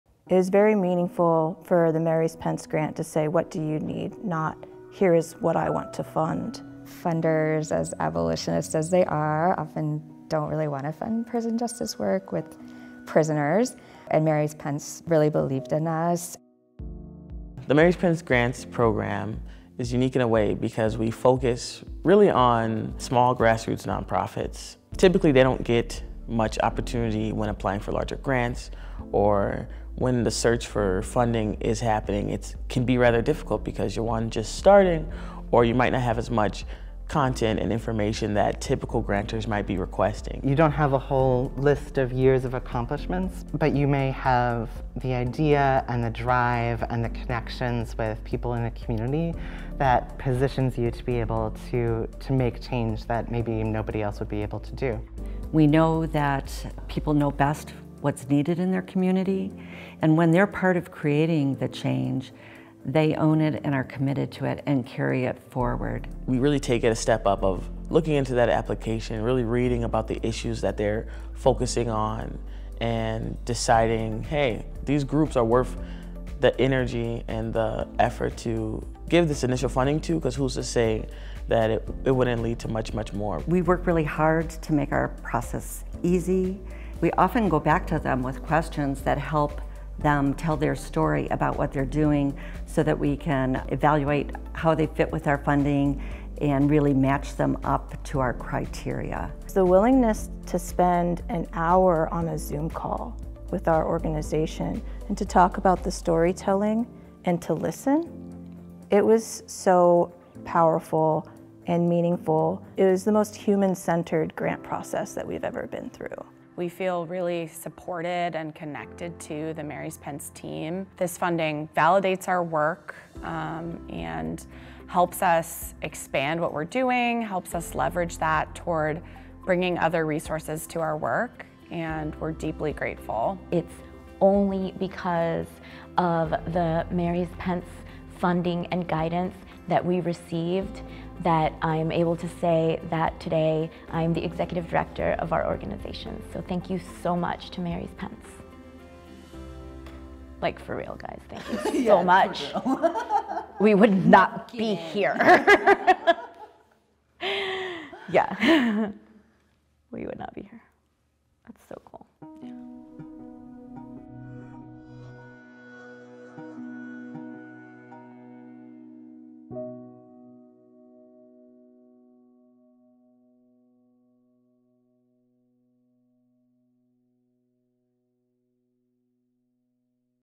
Mary's Pence Grants Program
Learn about the uniqueness of Mary's Pence Grants in the words of our Grantees.